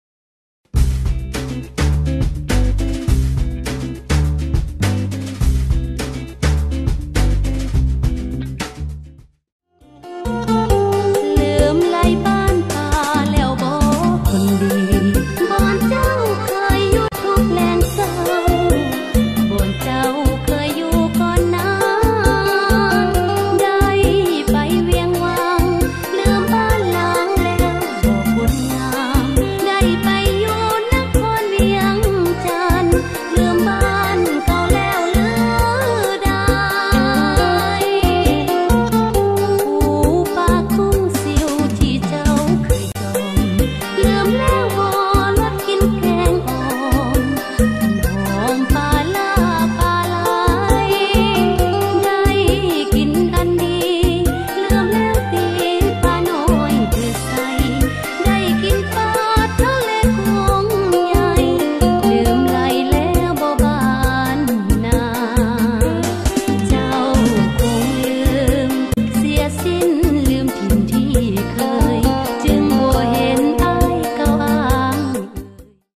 Vang Vieng : Walking to Pha Ngeun in Laos - Virtual Trip
Leaving the center of Vang Vieng in the north of Laos, you are quickly immersed in a deep typical countryside, with its karst peaks, caves and rice fields.
On the way to the Blue Lagoon, the tourist attraction of the corner, a few kilometers after the bridge of the village, you can find a view point from a road which goes on the right : it's the Pha Ngeun view point